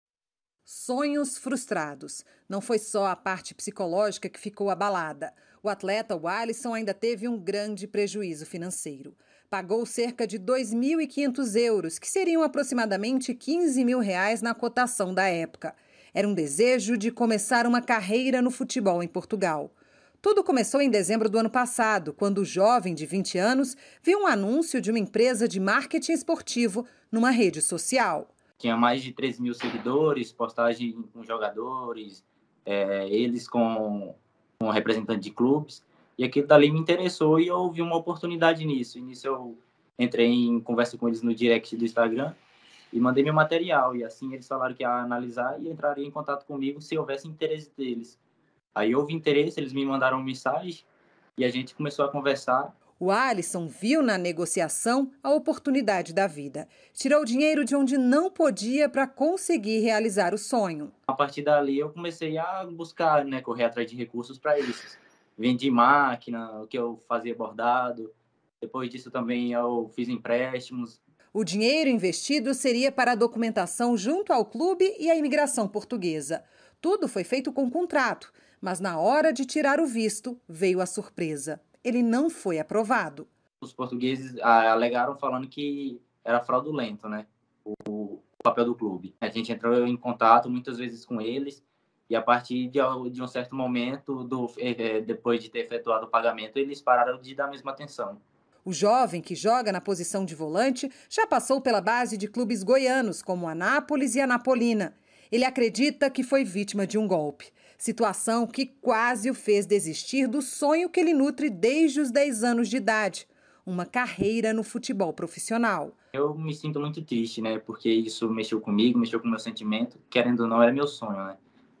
GOLPE DO FUTEBOL: JOVEM PAGA R$ 15 MIL E SUPOSTO AGENCIADOR SOME
A POLÍCIA INVESTIGA UM SUPOSTO GOLPE CONTRA UM JOGADOR DE FUTEBOL. O JOVEM TERIA PAGO 15 MIL REAIS PARA UMA FALSA AGÊNCIA DE MARKETING ESPORTIVO. OS GOLPISTAS PROMETERAM QUE LEVARIAM O JOVEM PARA JOGAR BOLA NA EUROPA. MAS,  DEPOIS DO PAGAMENTO, DISSERAM QUE ELE NÃO TINHA SIDO ACEITO PELO CLUBE E SUMIRAM.